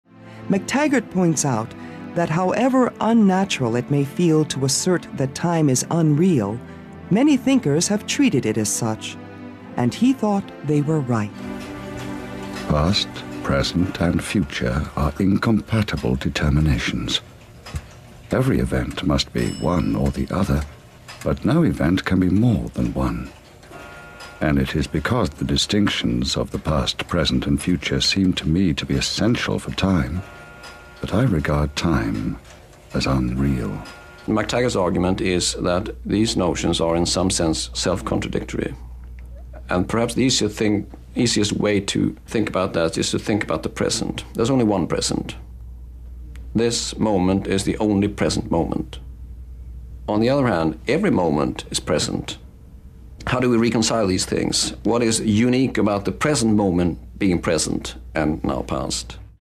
The Unreality of Time (McTaggart's Argument)
This is a clip on a famous argument by John McTaggart against the reality of time. This comes from a program about time a number of years back.